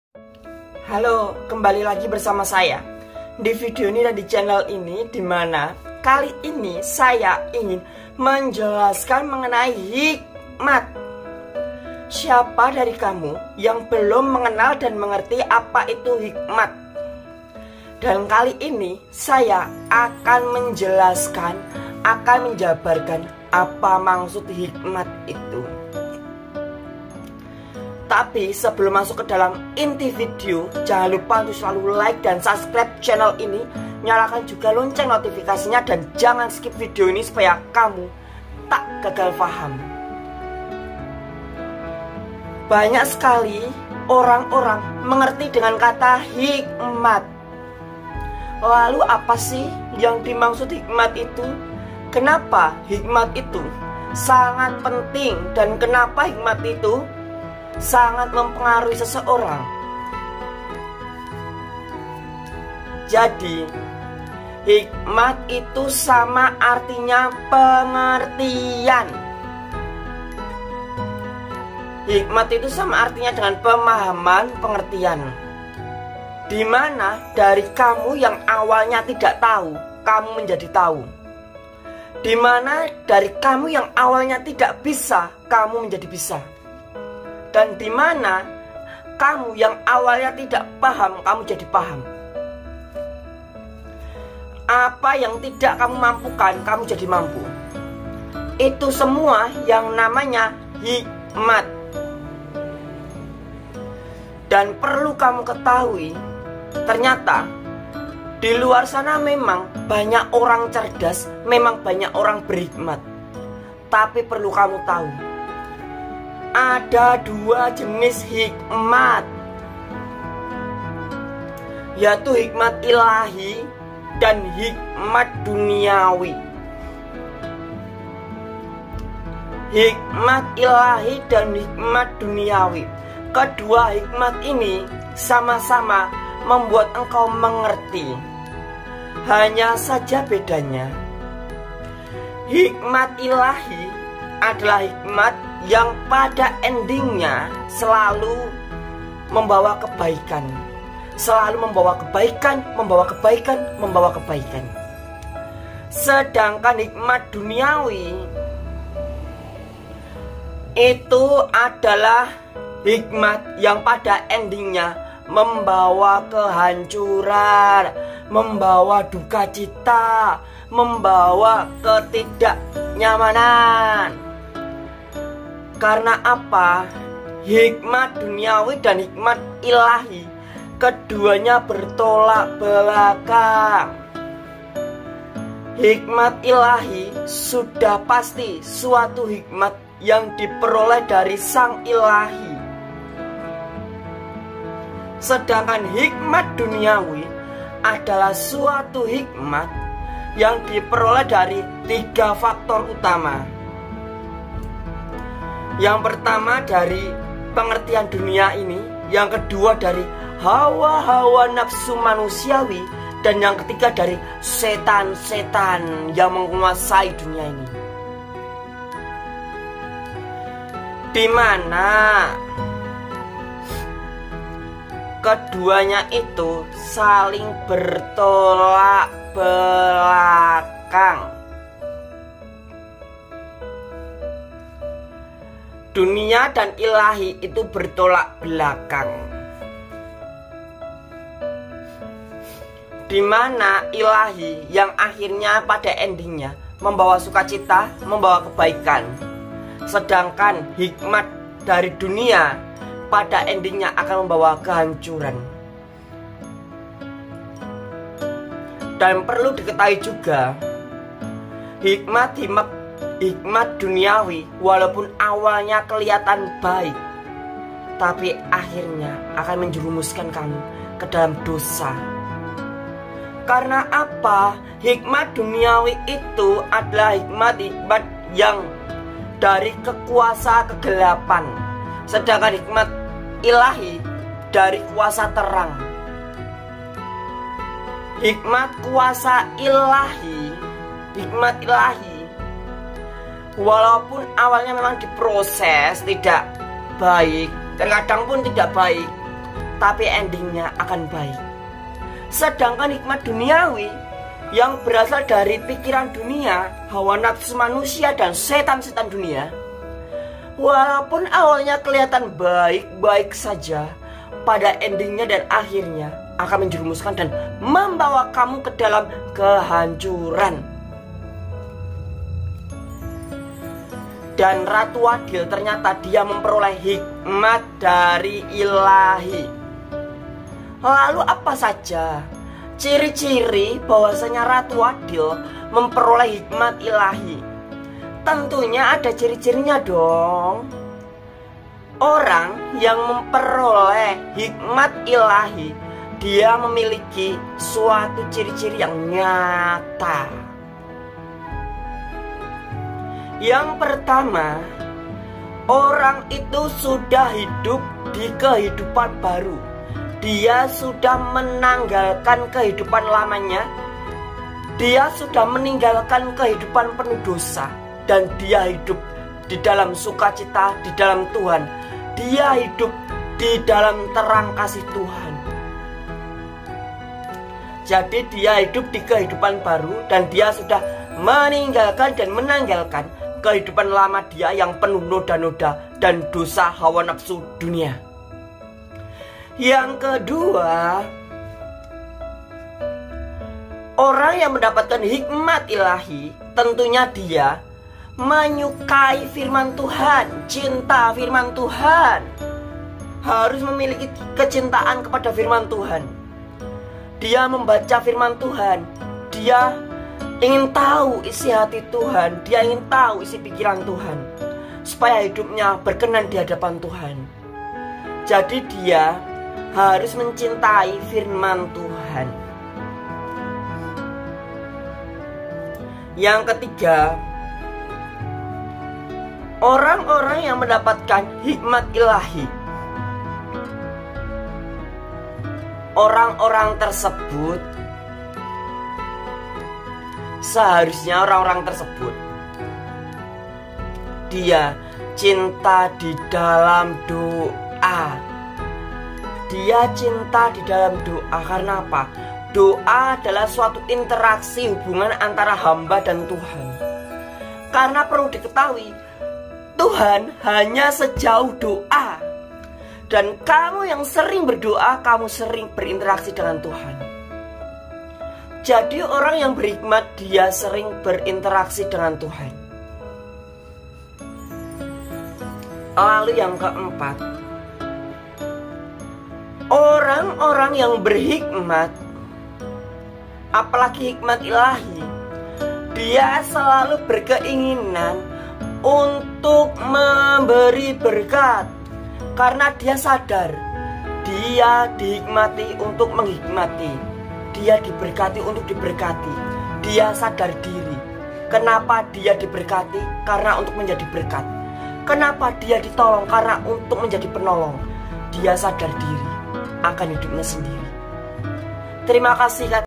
Ratu Adil Berhikmat Ilahi & Ini Ciri Cirinya
Vidio ini menjabarkan mengenai sosok ratu adil yang memperoleh hikmat ilahi.